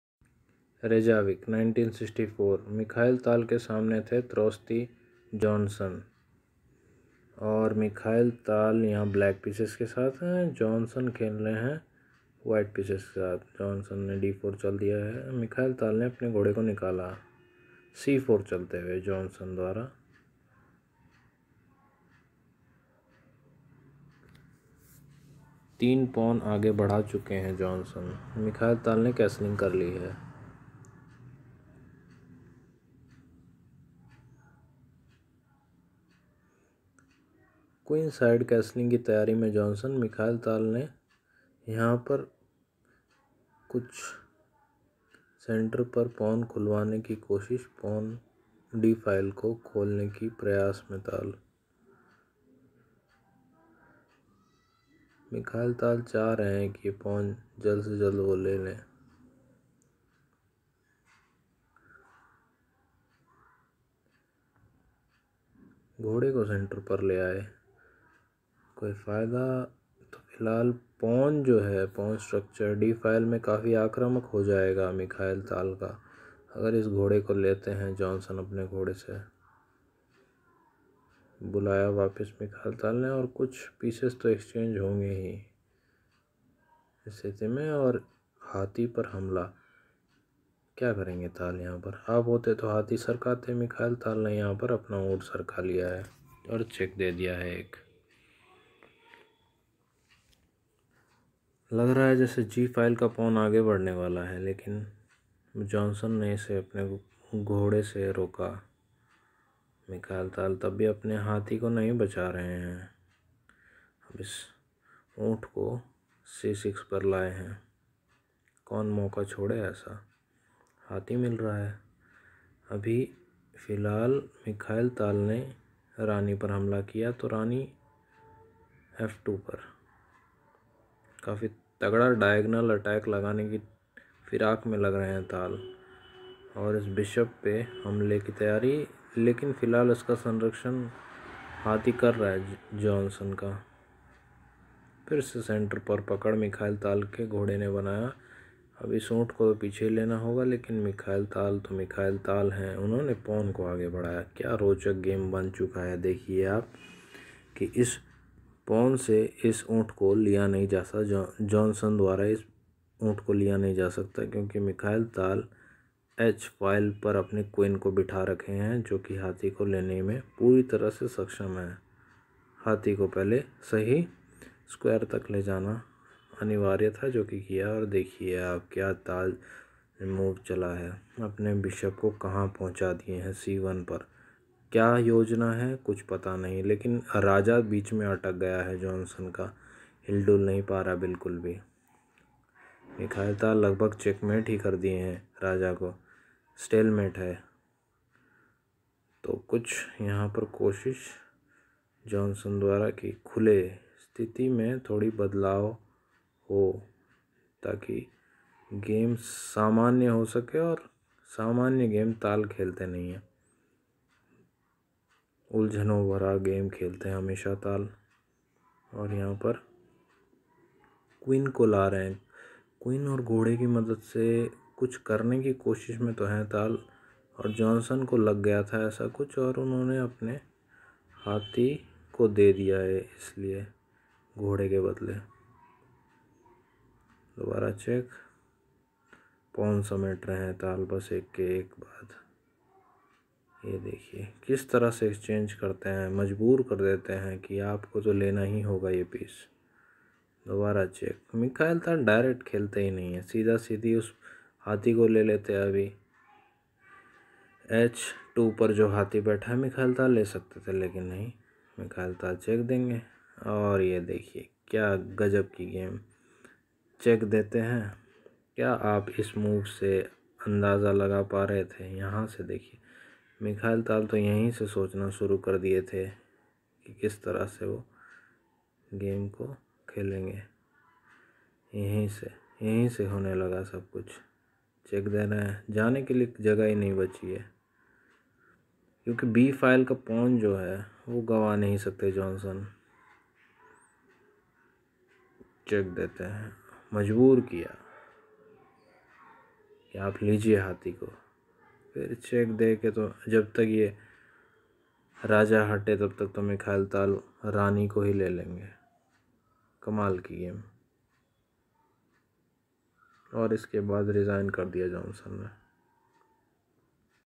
Mikhail Tal Vs Trausti Bjornsson 1964
This video is about a game played by the Legendary Mikhail Tal who was regarded as the "Magician from Riga"

Mikhail Tal
Magician from Riga
talHindi chess